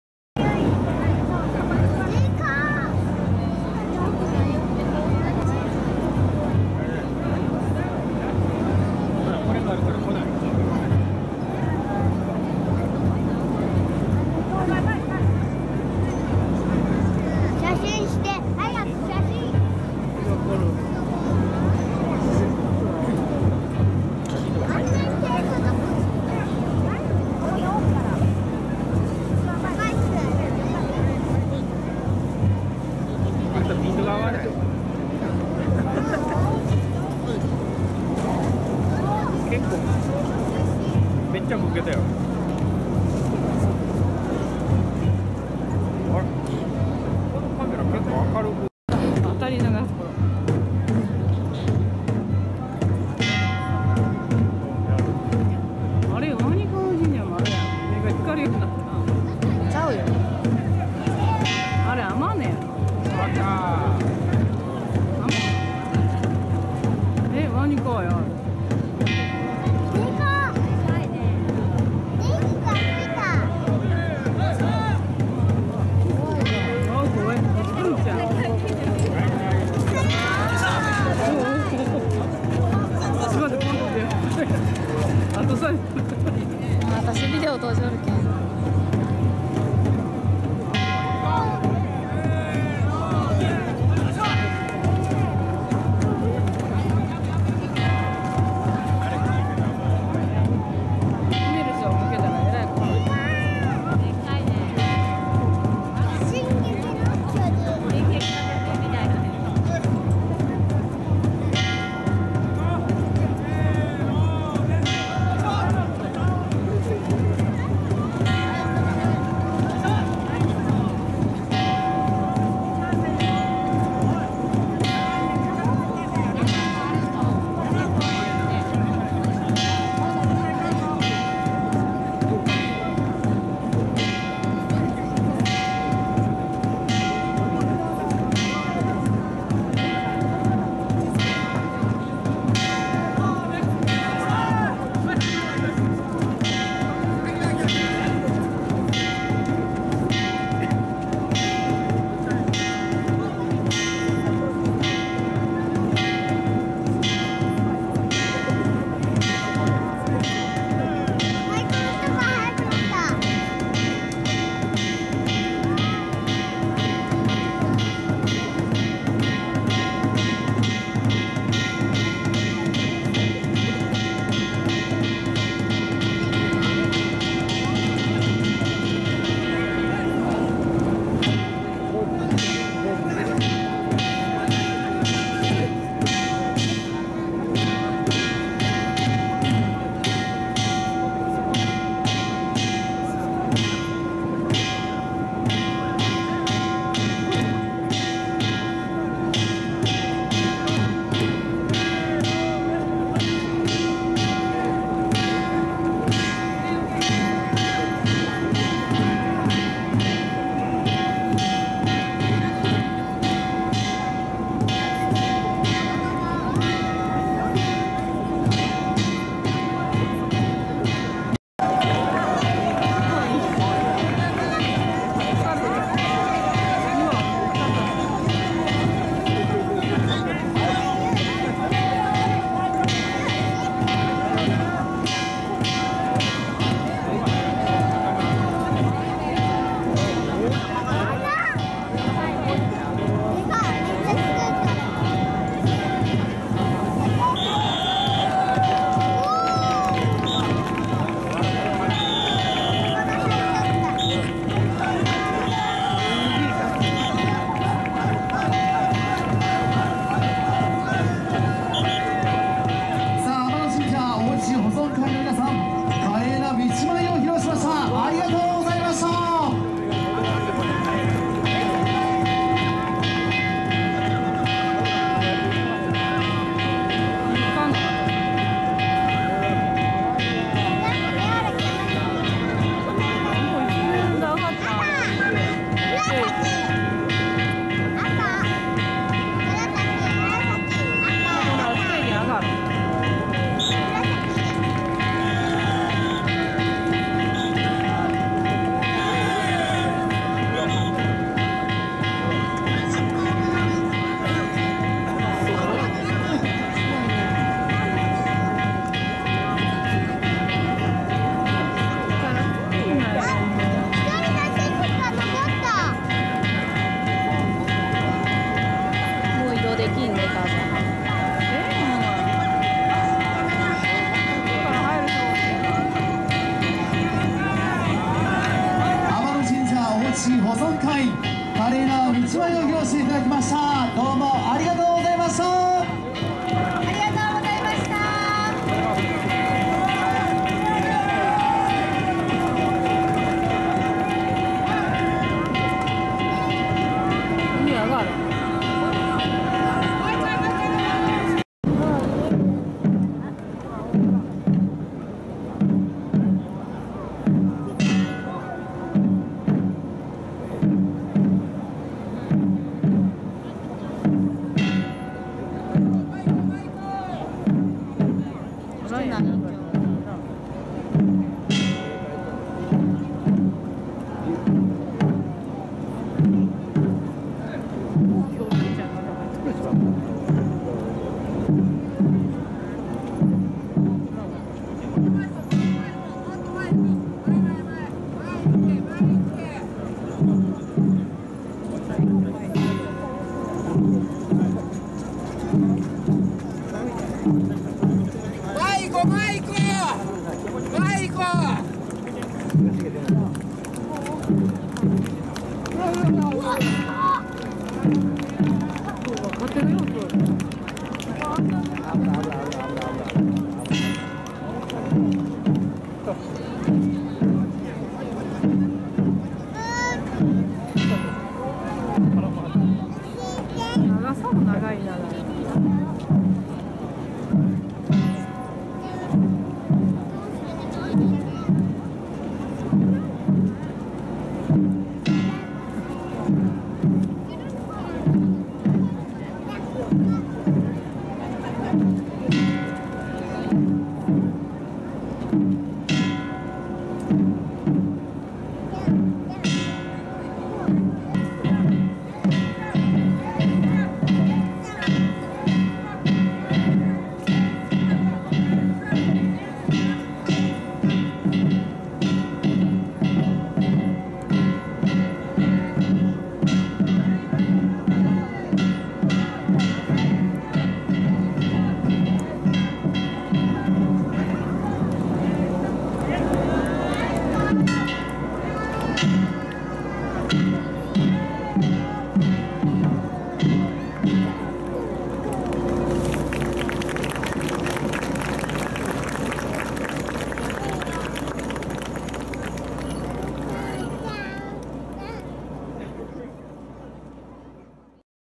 【まんで願大獅子】3体の違いと迫力の演舞！あなたが好きな獅子はどれ？
三木町の3体の大獅子動画。大きさやデザイン、舞い方などそれぞれ全然違うので見ていて楽しい。子獅子ほどバリエーションは多く無いものの、迫力が違いすぎます。
一番大きい天野神社は28mもあるのでカメラにおさまり切らない。
四条鰐河は小さいボディを活かしてアクティブな演舞ができます。